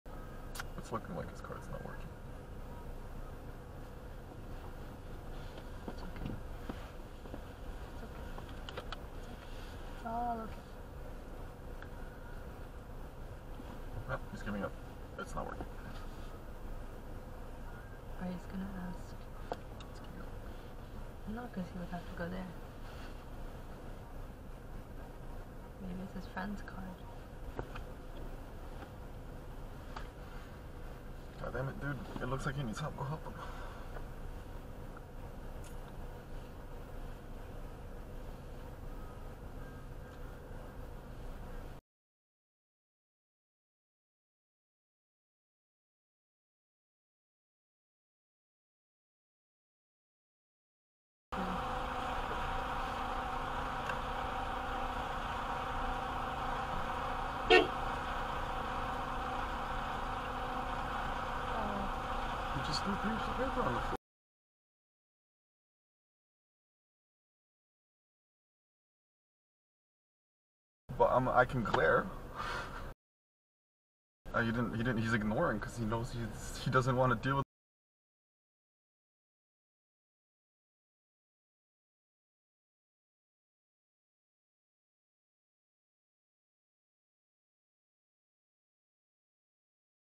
The World Is My Garbage
While waiting for gas we encountered this specimen. I mean I could rationalize all day and say it doesn't matter really but it mattered enough for me to upload this... The glare I gave this dude...

A person taking the receipt from the previous customer and trashing it in the wind. He ignored getting honked at, I mean who would dare honk at him...

Like the drive, driving, commentary, video or music? Have any suggestions? Please let us know!